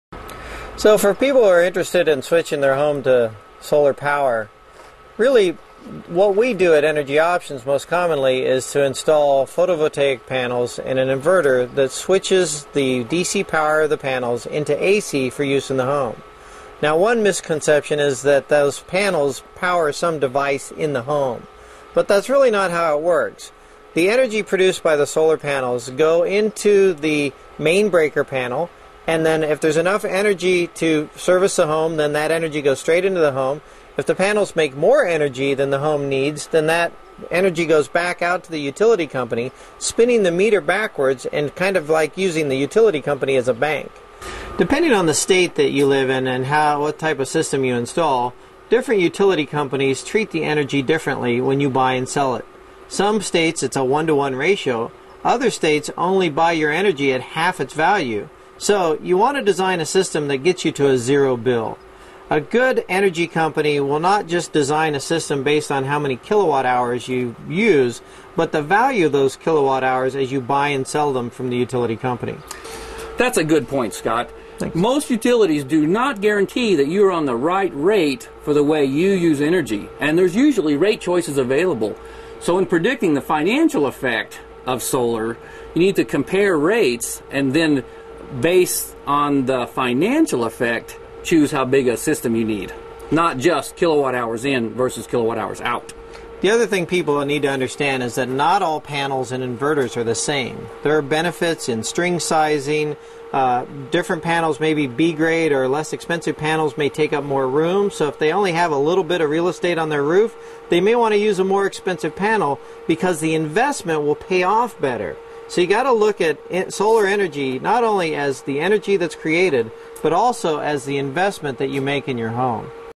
Switching to Solar Power for your home
for more solar panel information.

Converting from DC to AC and more general solar power information.